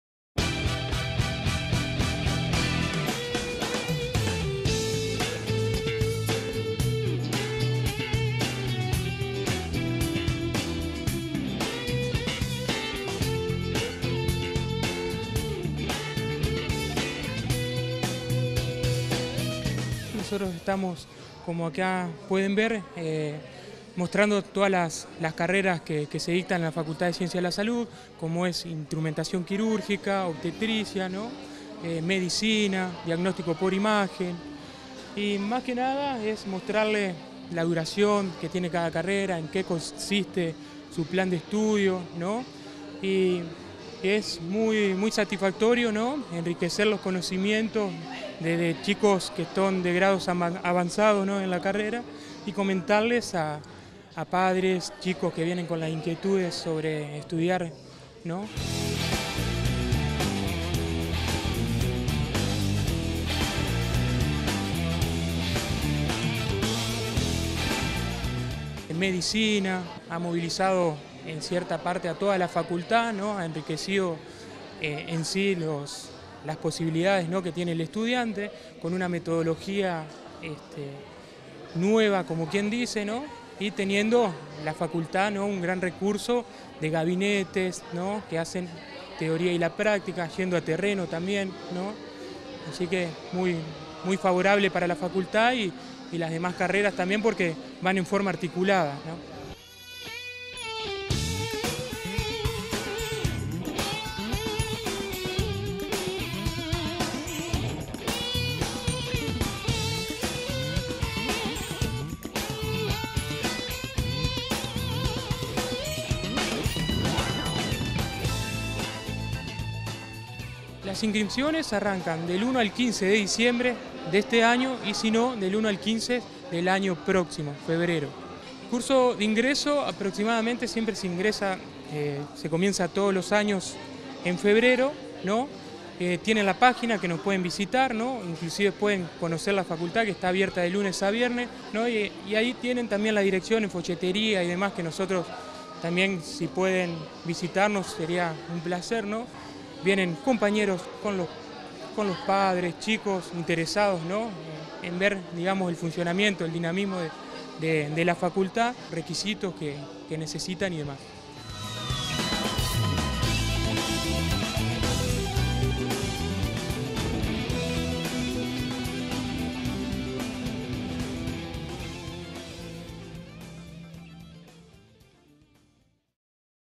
Feria de carreras en Concepción del Uruguay 2016
Como parte de la actividad de difusión de la propuesta académica y de servicios de nuestra Universidad, personal de la Secretaría de Extensión Universitaria y Cultura realiza charlas y participa en ferias en distintas localidades, destinadas a estudiantes de los últimos años del nivel secundario.